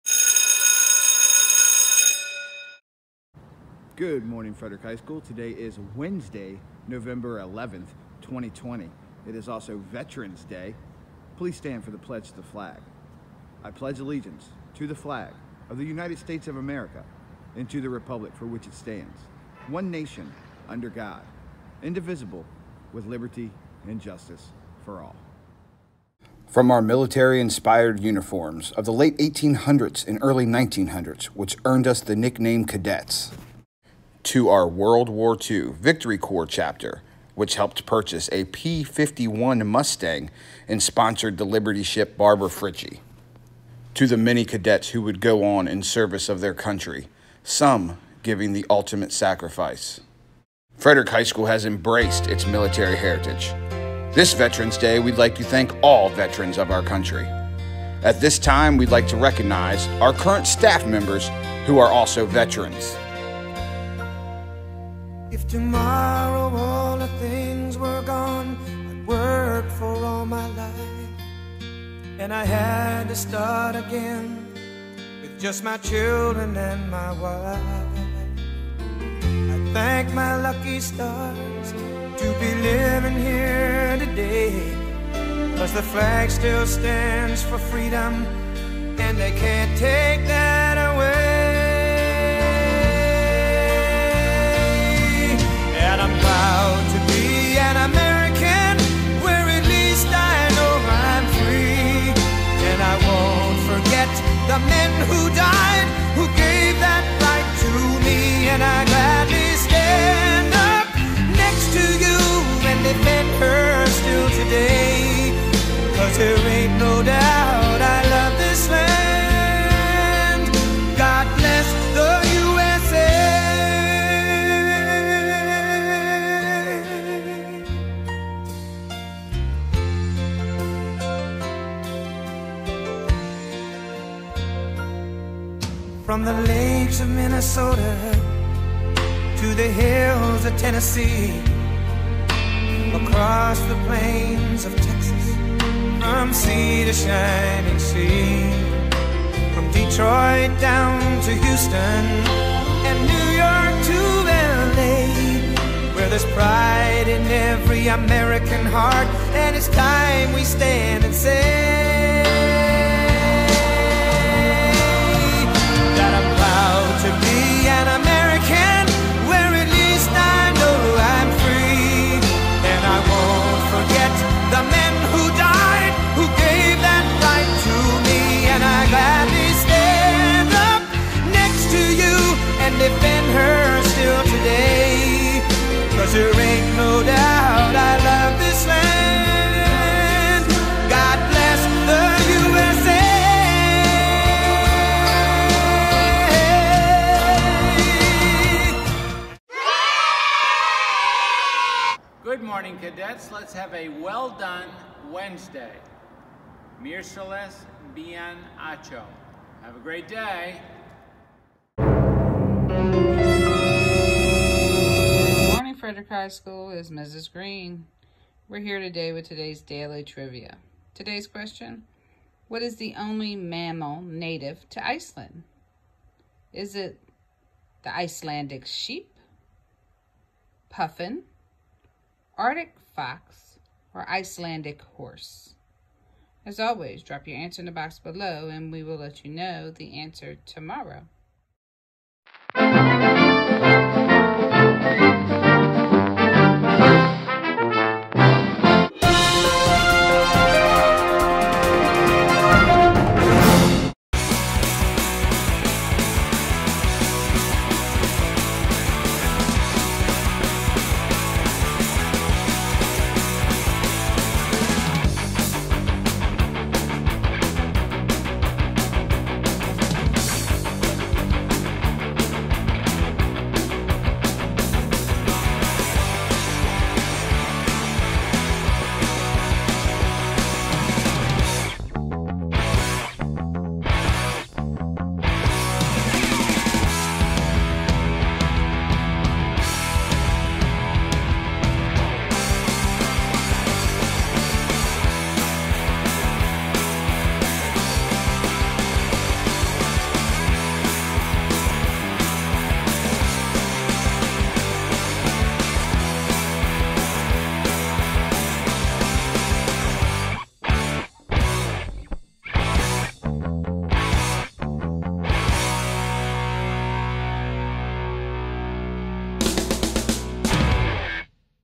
Wake Up FHS⏰ 11/11/2020
Frederick High School’s morning announcements for November 11th 2020.